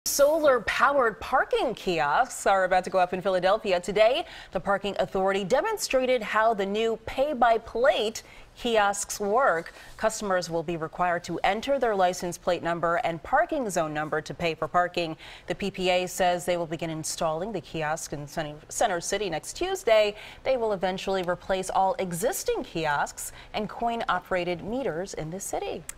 PPA Solar-Powered Kiosks To Replace All Parking Kiosks In Philadelphia
The PPA says they will begin installing the kiosks in Center City next Tuesday.